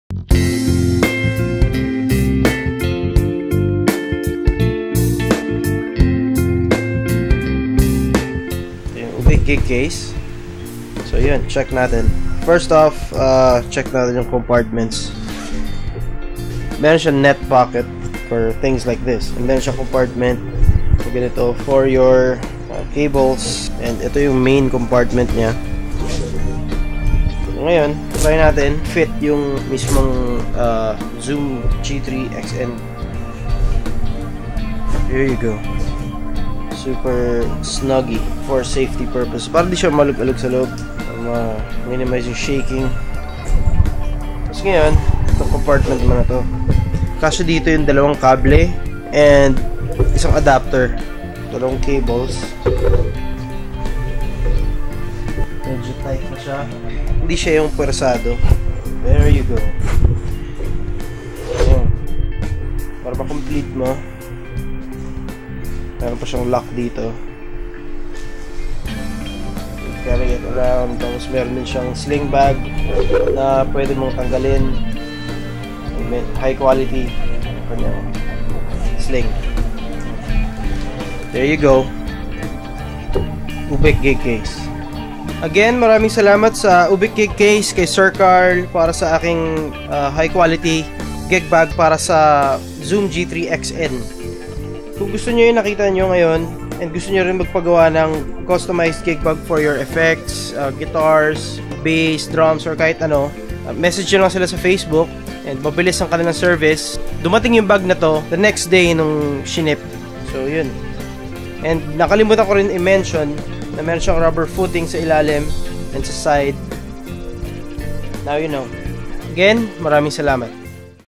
UBEC GIGCASE (Gigbag for Zoom G3Xn Review)
I would like to thank Carl Delos Angeles Geverola of Ubec Gigcase for my two gig bags. The first one he sent me was a gigbag for my Epiphone Les Paul way back in August 2017!

Here's a link to UBEC GIGCASE's Facebook Page so you can hit them up.